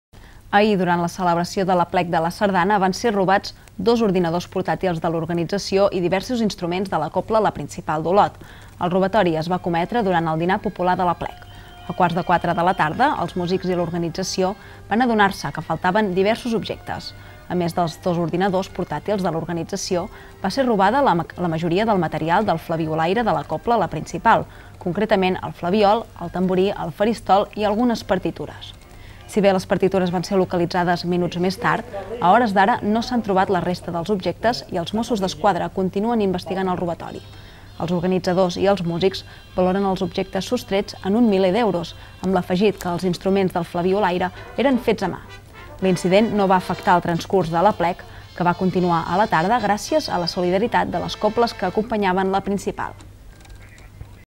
Roben ordinadors i instruments de la Cobla La Principal d’Olot durant l’Aplec de la Sardana
Ahir, durant la celebració de l’Aplec de la Sardana, van ser robats dos ordinadors portàtils de l’organització i diversos instruments de la Cobla la Principal d’Olot. El robatori es va cometre durant el dinar popular de l’Aplec.